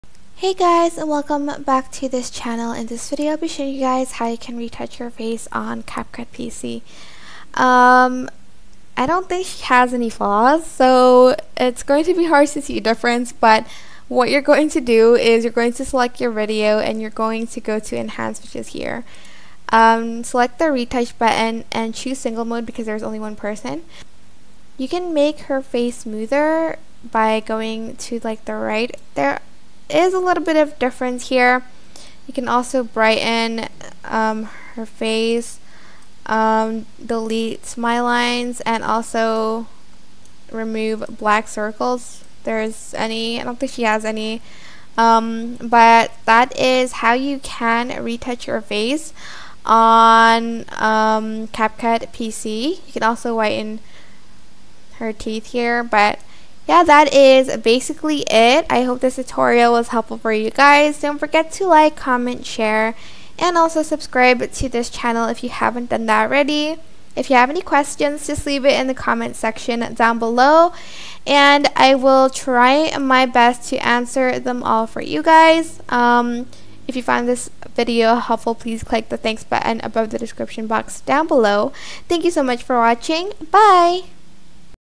How To Retouch Face In CapCut PC
Want to retouch your face on CapCut PC? Here's how you can do that.